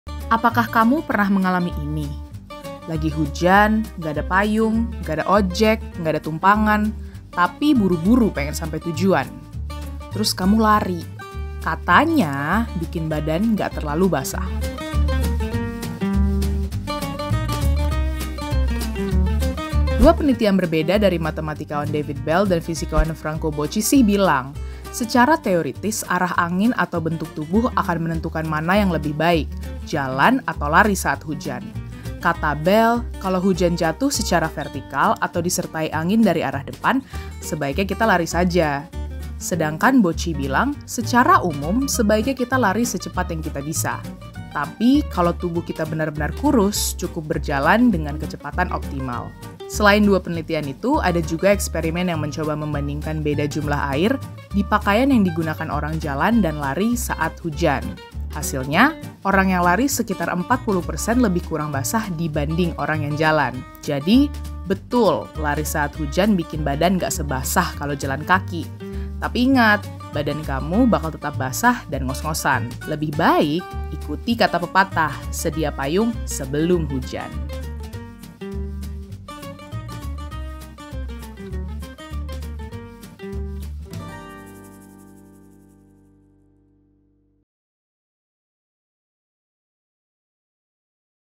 Kejebak Hujan Gak Bisa Berteduh, Mending Lari atau Jalan Kaki? | Buka Data
Kalian bisa tonton Buka Data ini sebelum berangkat atau pulang kerja. Biar kalau kejebak hujan (plus gak bisa berteduh; enggak bawa payung; enggak ada tumpangan), kamu enggak bingung lagi mau pilih jalan kaki atau lari.

(Narasi)

Tonton juga Buka Data eps. [Kejebak Hujan Gak Bisa Berteduh, Mending Lari Atau Jalan Kaki?] dan episode lainnya di